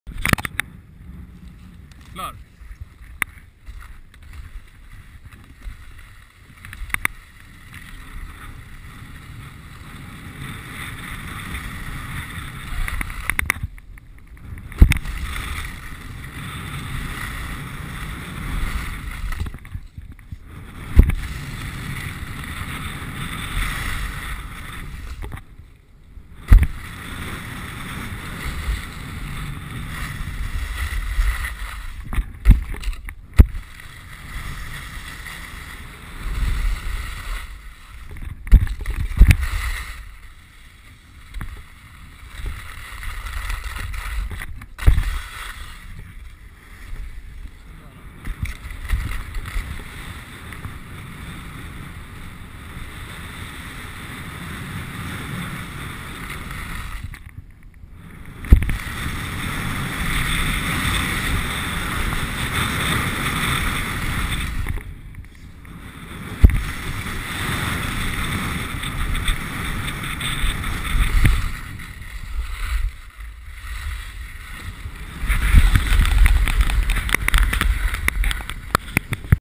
Klövsjö Funpark 2012.MP4
Skidfilm från Klövsjö - en genomkörning av funparken.